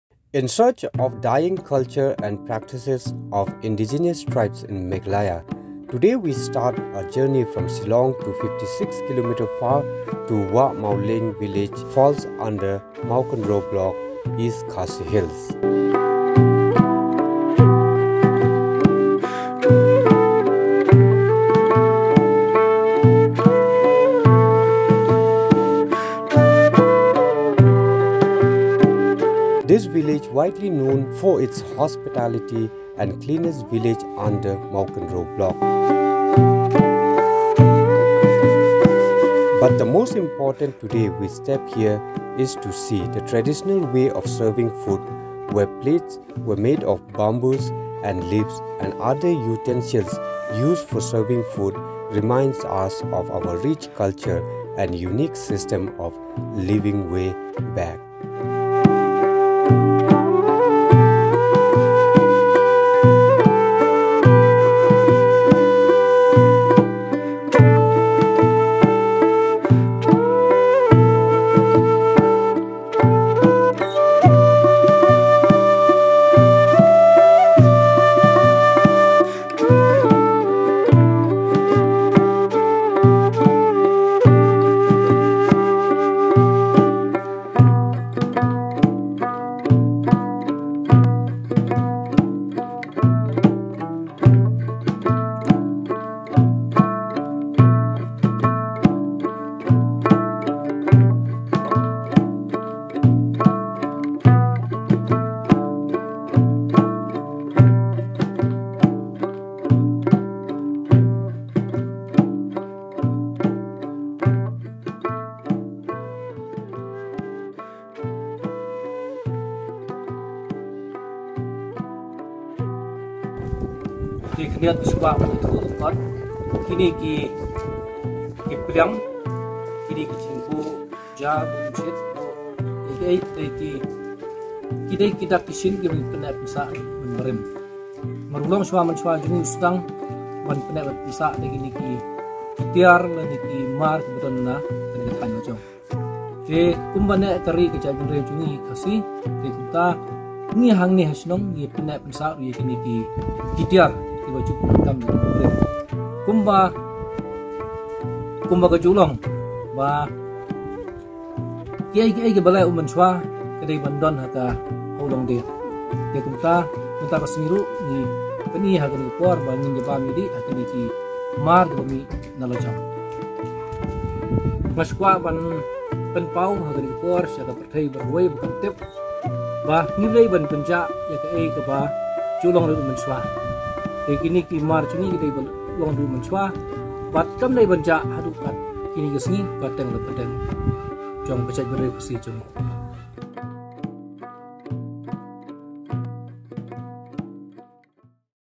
Wah Mawleiñ Village ( A framework to protect cultural practices) A documentary
Wah Mawleiñ Village ( A framework to protect cultural practices)